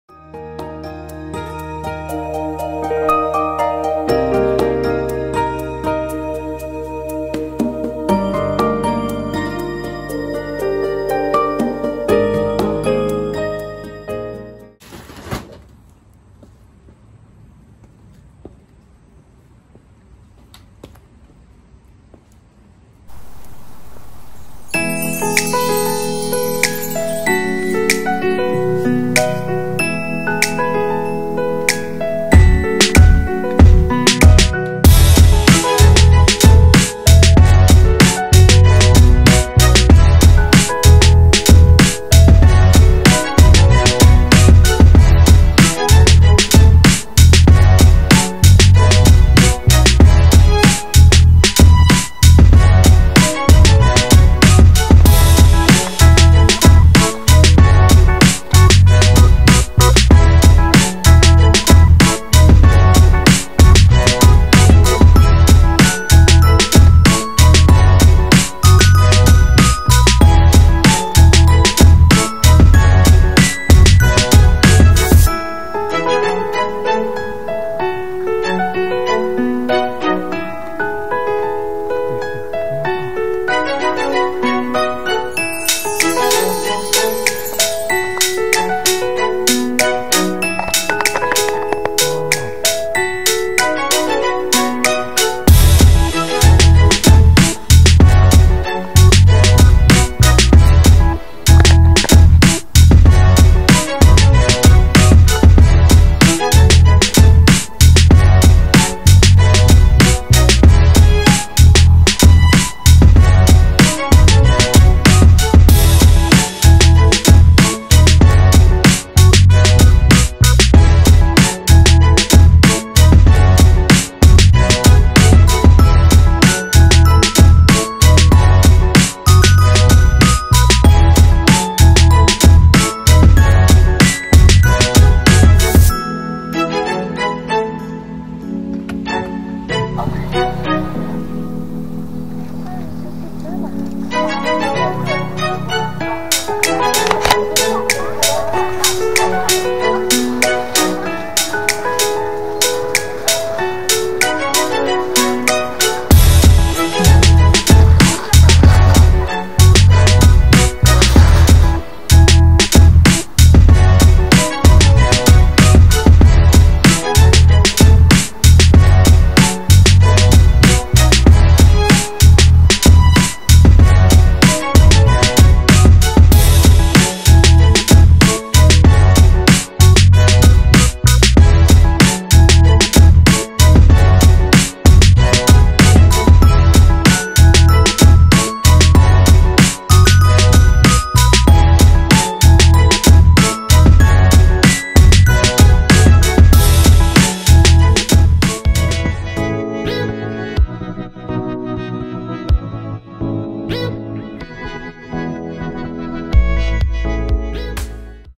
片麻痺の日常、雨の日の買い物。片麻痺YouTuberの会、vol.70
雨の日でも、小降りのチャンスを狙って買い物しています。